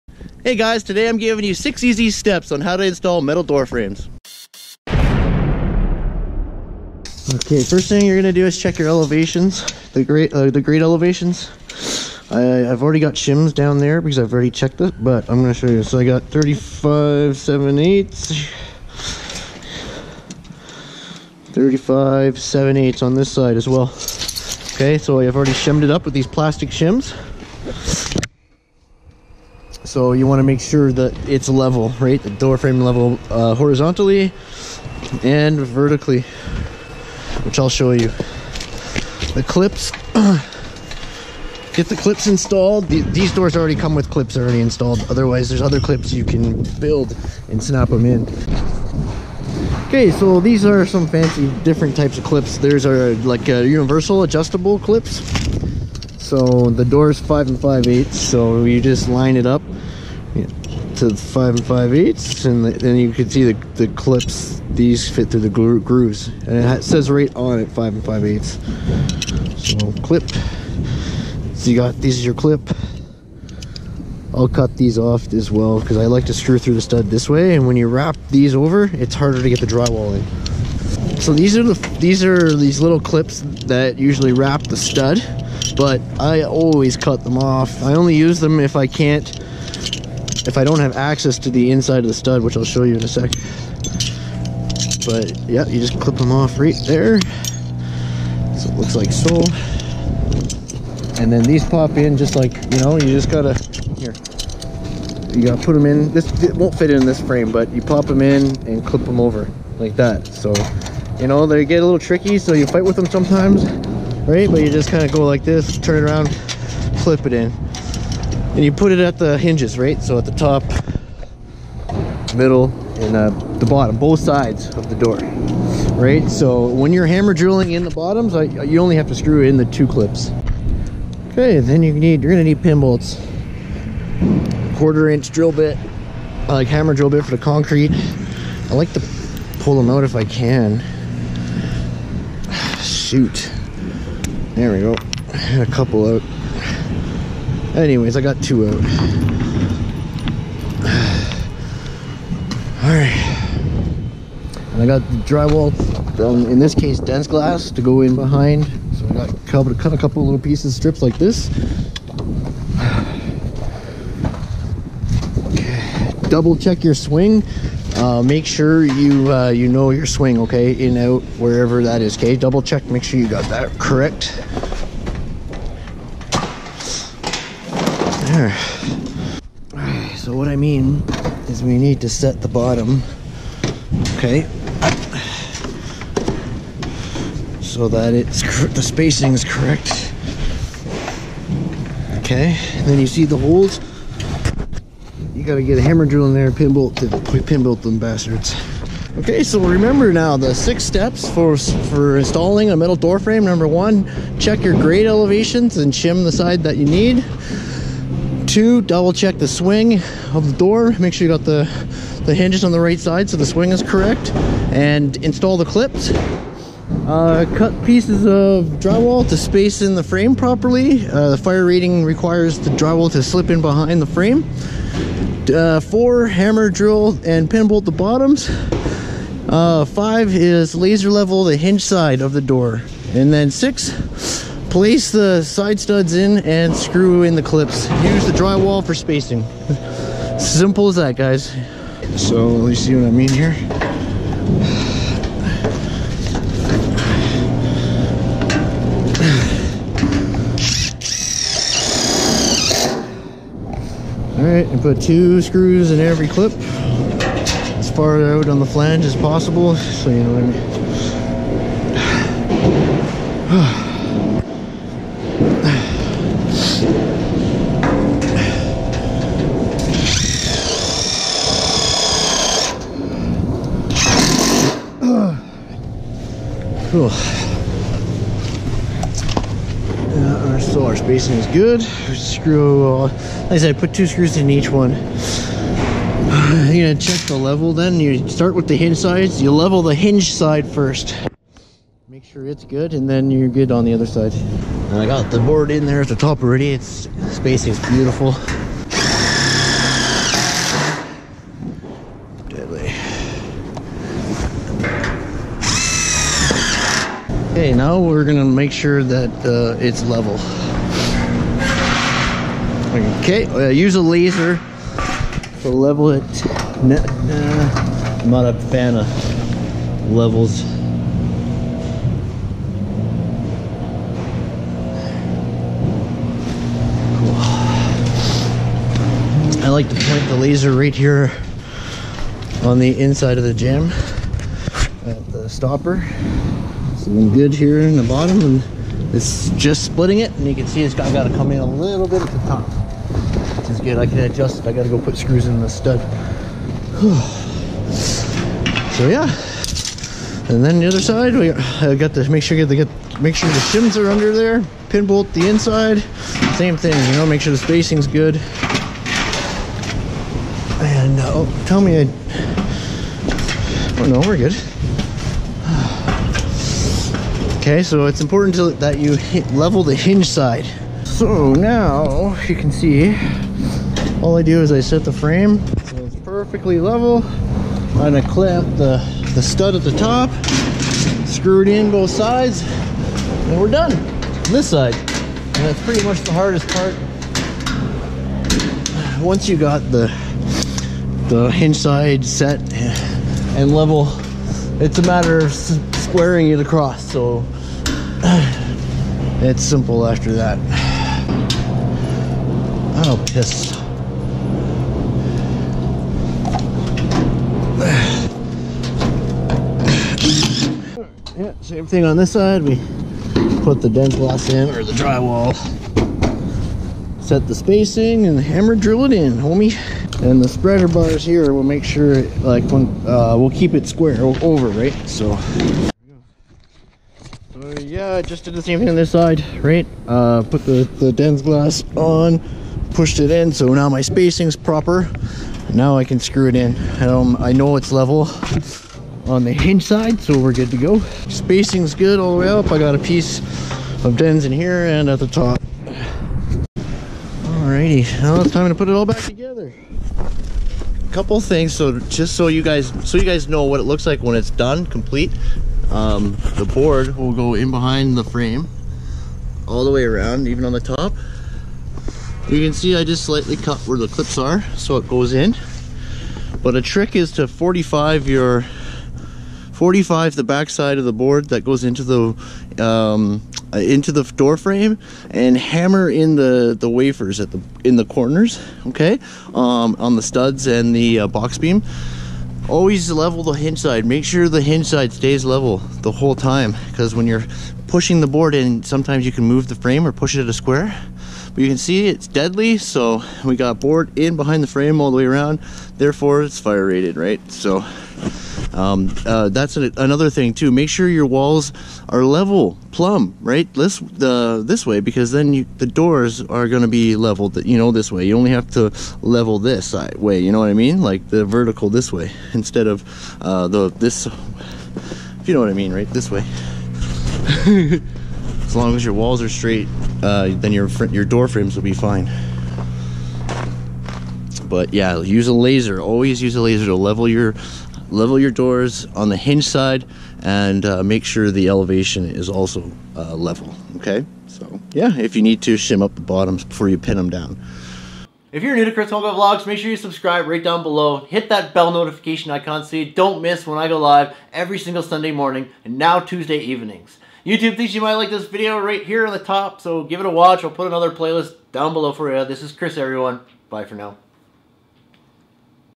Installing A Metal Door Frame In 6 Easy Steps
Installing a metal door frame can be done in 6 easy steps. These are those steps. MORE on how to install a steel door frame

🚧 Wanna See More?

✅Scan, File &, Go Paperless.
Coming to Android Soon!

✅ My Tool Recommendations & YouTube Gear

💥Schedule

Chris Konkle.